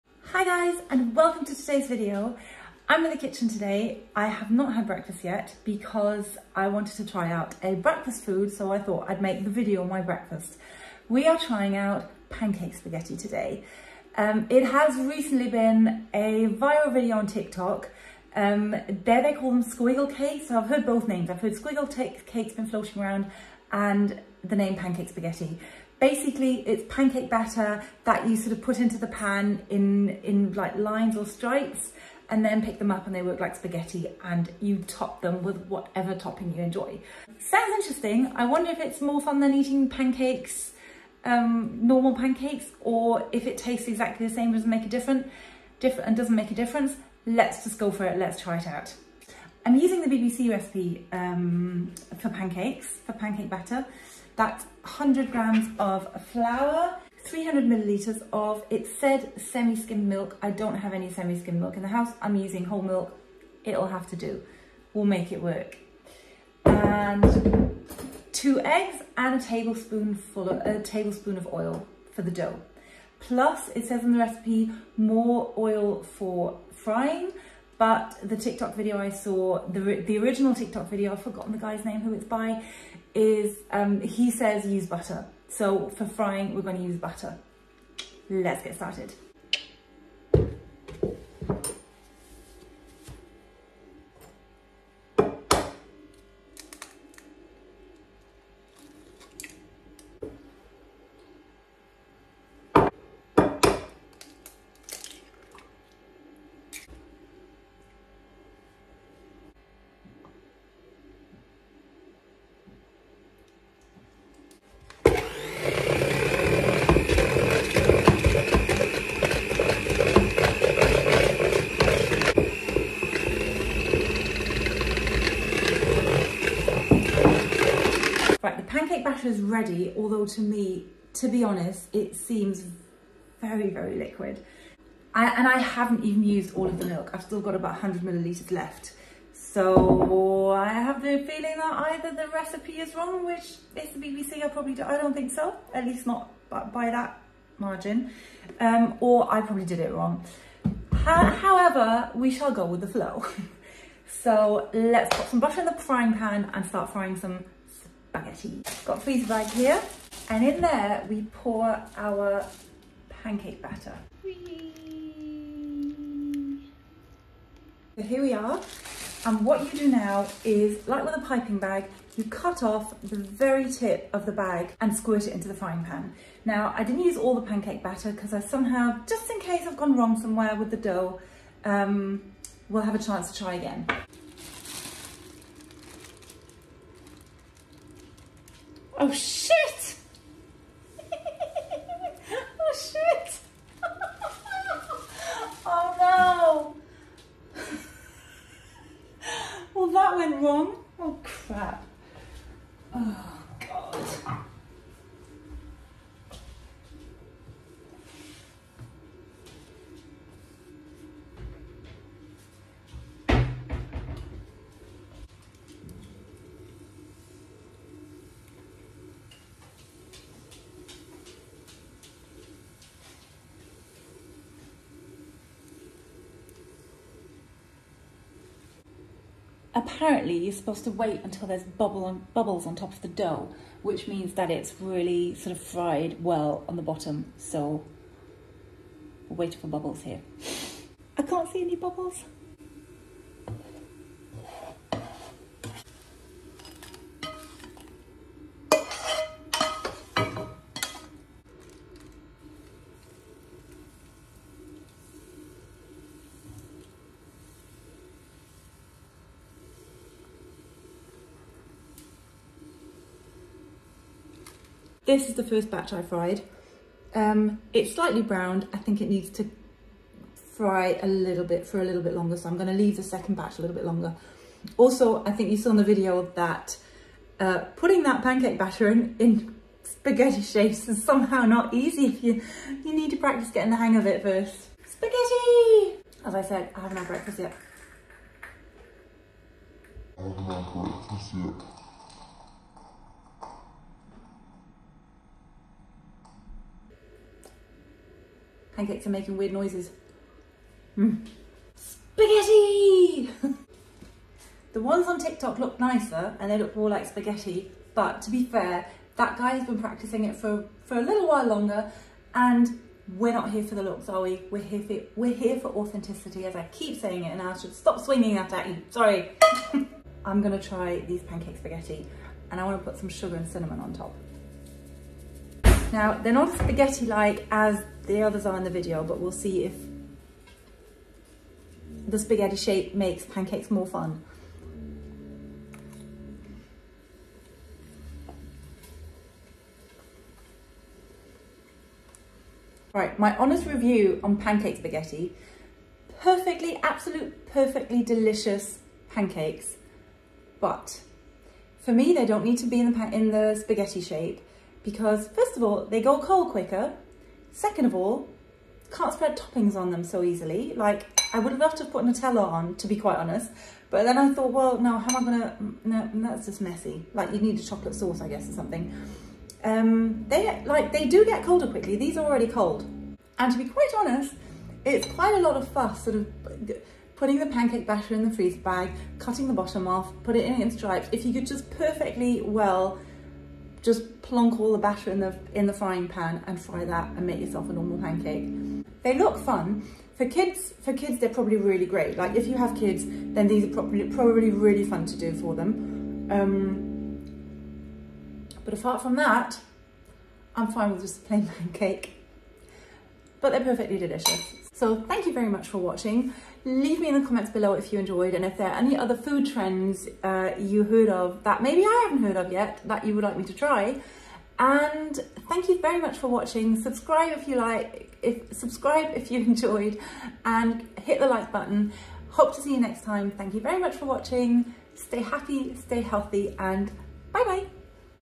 I tried the TikTok trend pancake spaghetti
I tried the TikTok trend pancake spaghetti- are they any good….?